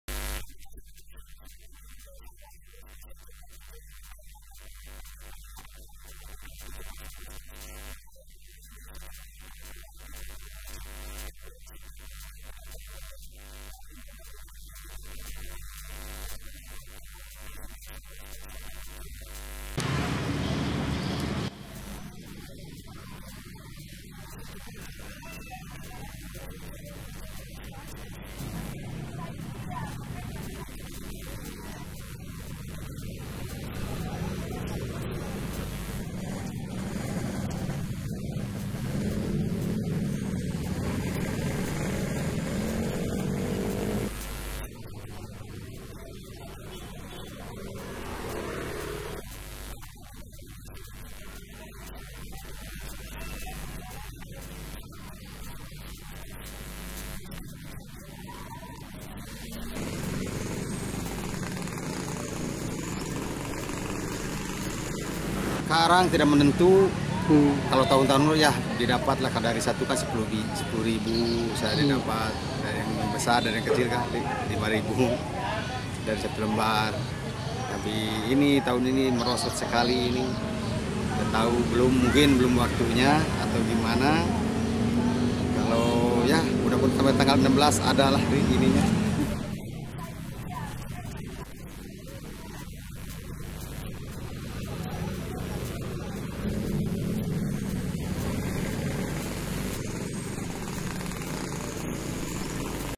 Pernak Pernik HUT Kemerdekaan | iNews Sulbar 02-08-2024
Awal agustus, sejumlah penjual bendera dan pernak-pernik perayaan hari kemerdekaan 17 agustus mulai ramai di sejumlah trotoar di kota mamuju. Hampir setiap tahunnya, pedagang bendera dari berbagai daerah ini datang ke kota ini, memanfaatkan momen 17 agustus untuk berjualan.